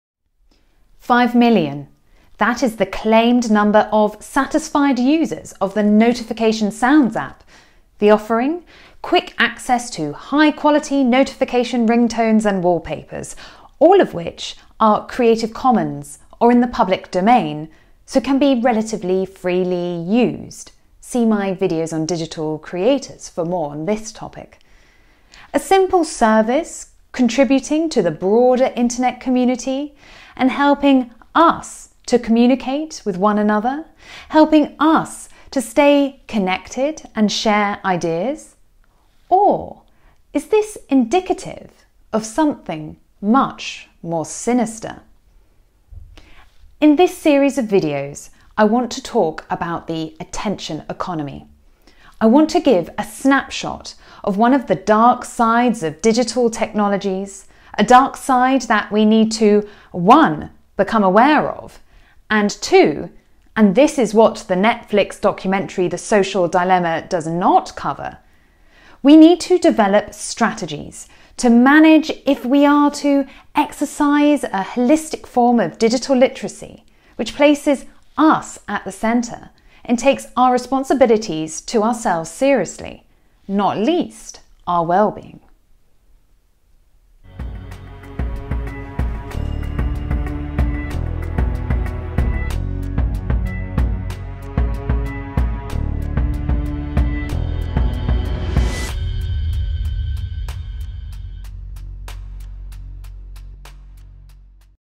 15. Introducing the Attention Economy
This video sets the scene for a series of videos interrogating 1) what the ‘attention economy’ is and how it functions and 2) what we can do to fight back against it.